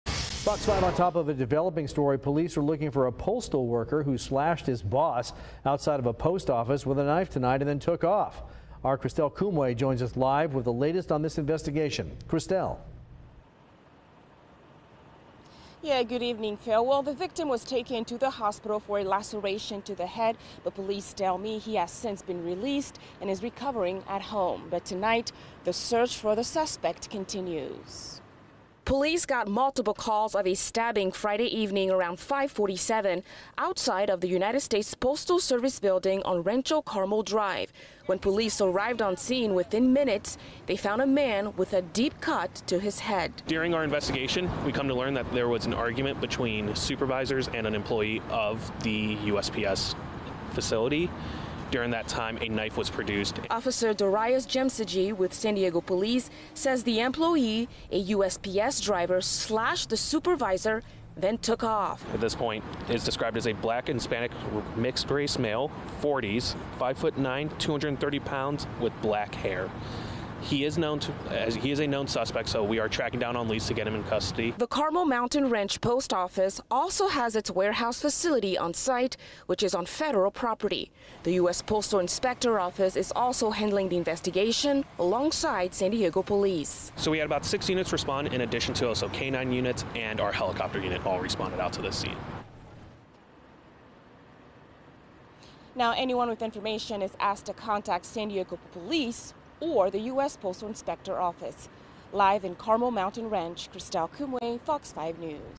Postal Office Driver Stabs Supervisor
Christelle Koumoué has the latest from Carmel Mountain Ranch.